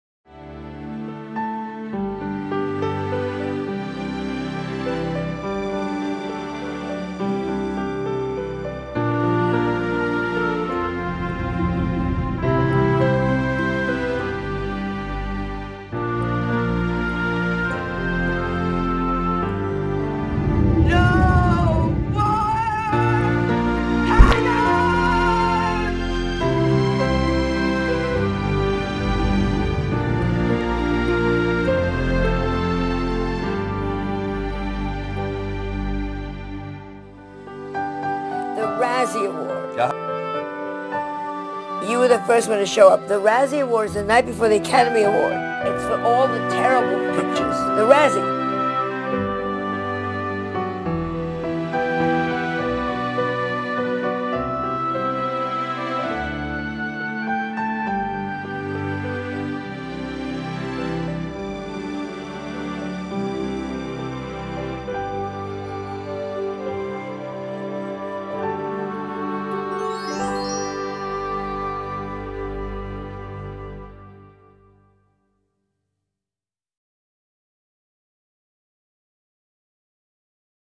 35th Razzies "In Memoriam"
Bob Hoskins, Frank Yablans, Richard Kiel, Joan Rivers, and Robin Williams were all great and we miss them even though some made a couple of Razzie-worth boo-boos!